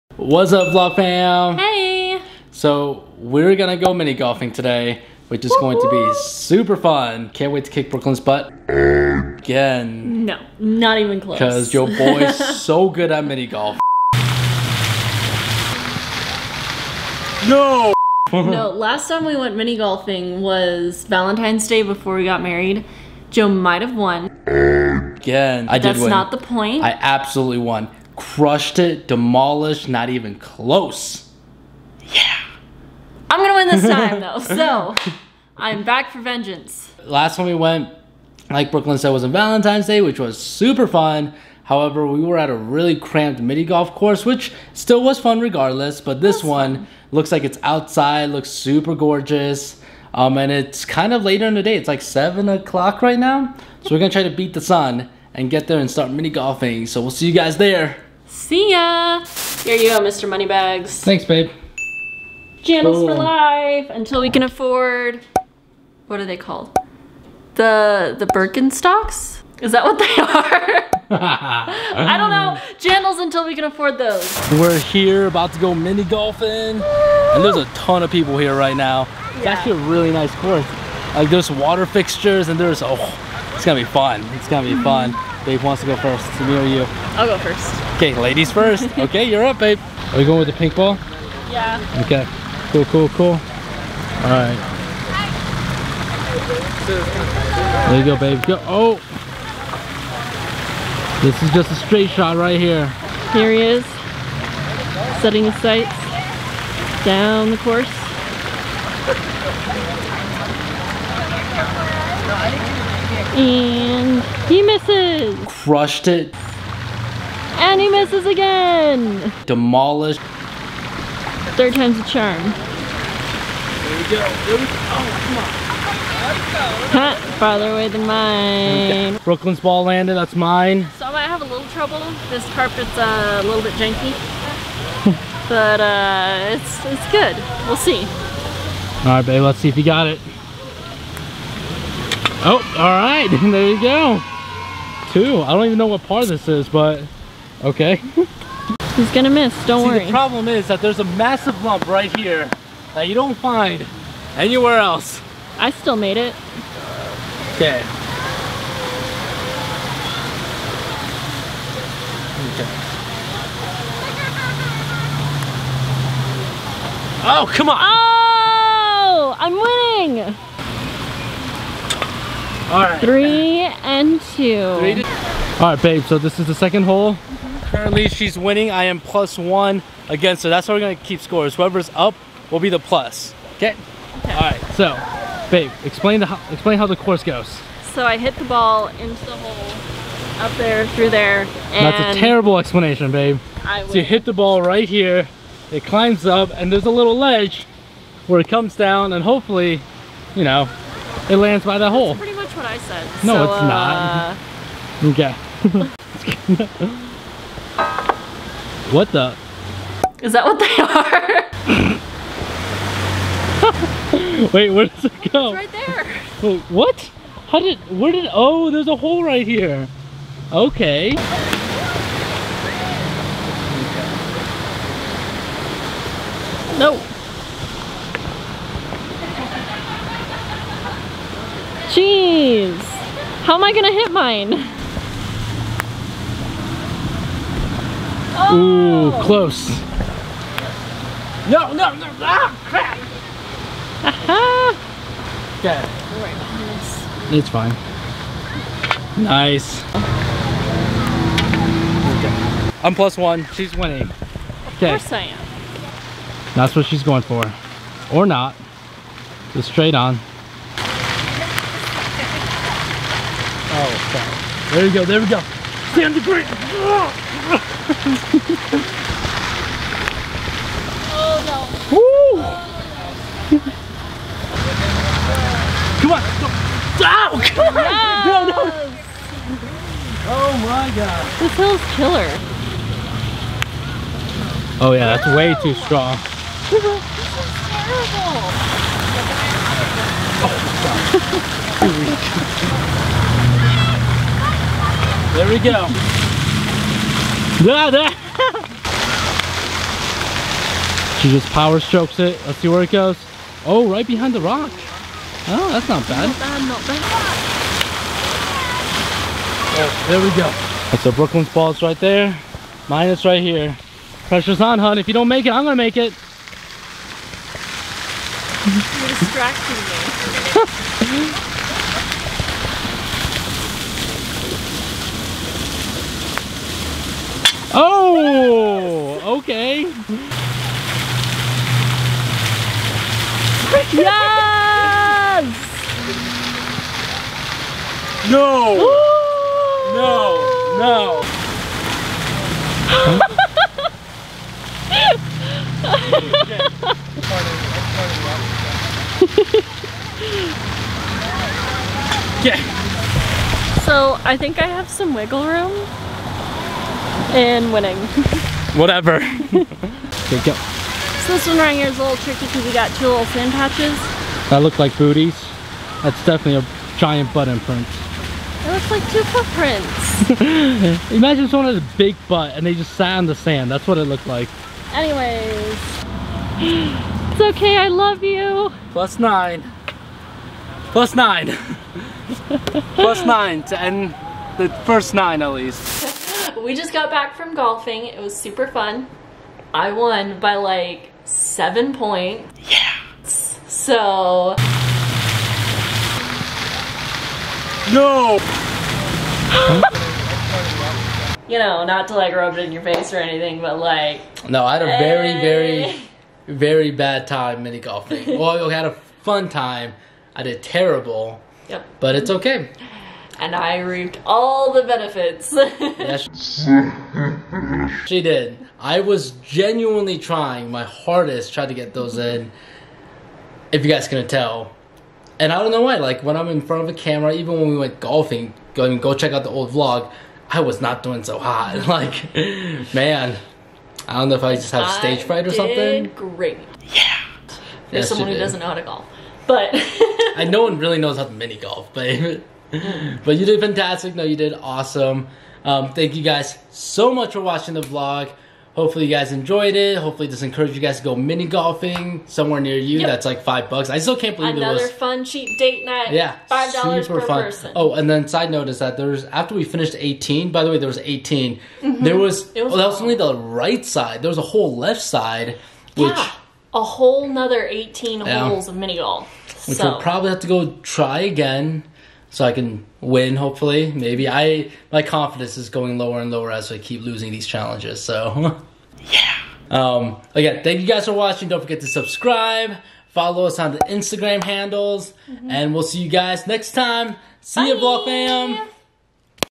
Sunset Mini Golf Date (WHO CRUSHED WHO???)
Whose the better mini golfer??? Now that's the question of the century! But... is it tho??? Either way, Who do you think will win? And go on a Cheap date to mini golf!

Seriously though, if you're looking for a SUPER FUN date, go mini golfing! Let's be honest, no ones really that good any ways, so it's a place you can go to just goof around and have fun for CHEAP!

We only paid 5 bucks each for 18 holes of mini golf! That's atleast an hour of fun! And being a first year married couple, we're always look for the BEST CHEAP date ideas!

Brooklin_dawn
Life_being_joe
BrooklinAndJoe